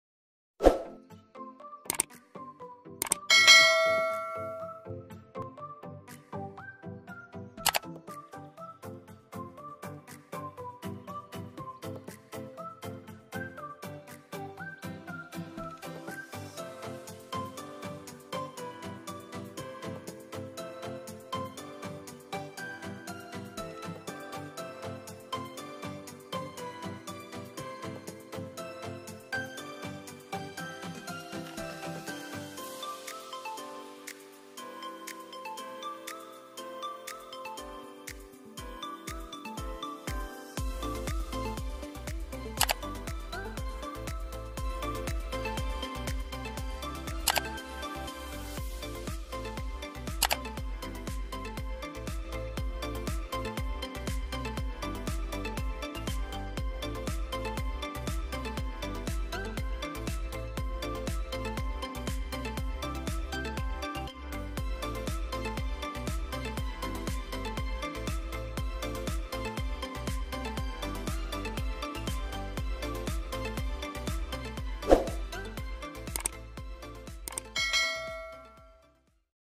HOW TO STOP IMAGE FROM MOVING IN KINEMASTER WITHOUT CROPPING
In this video, you will learn how to stop image from moving in your Kinemaster app without using cropping OFFLINE.